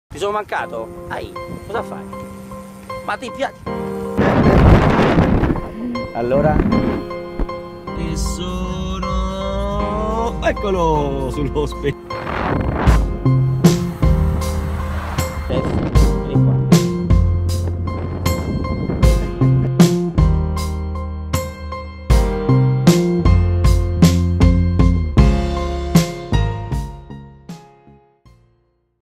La entrañable historia de Michele Scarponi y su amigo Frankje
Tras la repentina muerte de Scarponi su inseparable amigo sigue esperándole cada día.